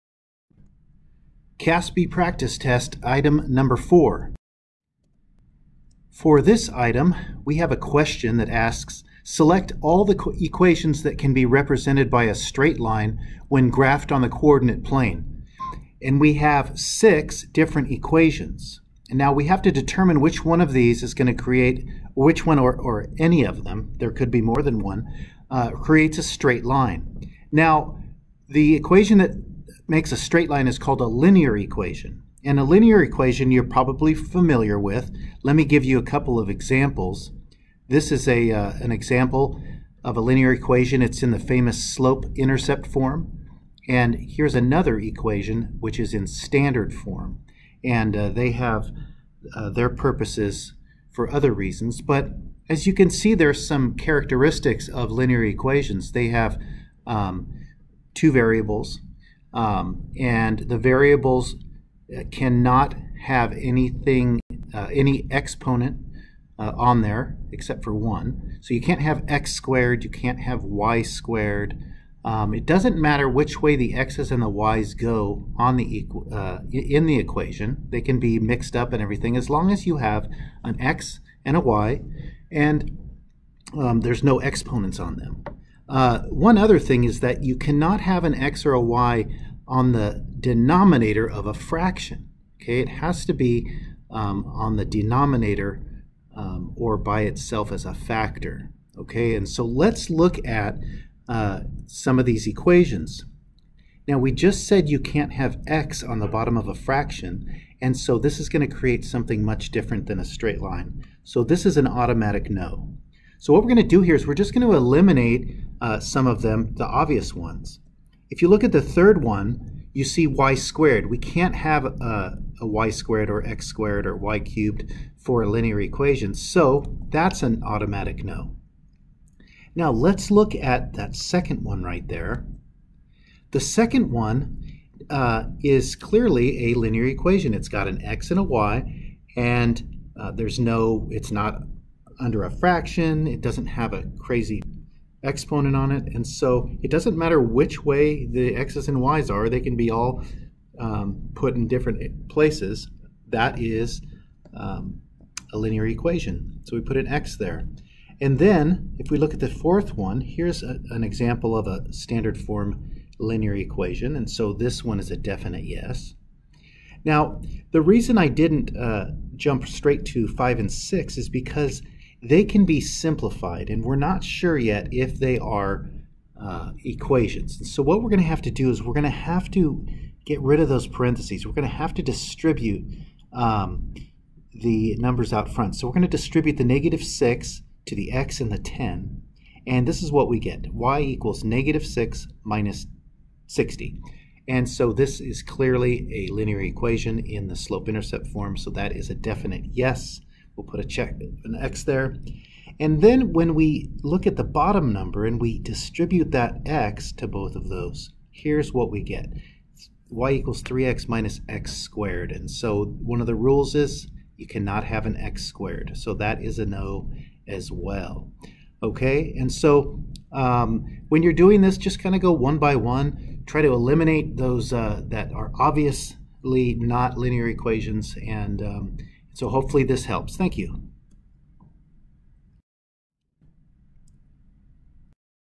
CAASPP Practice Test Item #4 8th Math Smarter Balance
This lesson shows how to do Item #4 on the 8th Grade Math CAASPP Practice Test.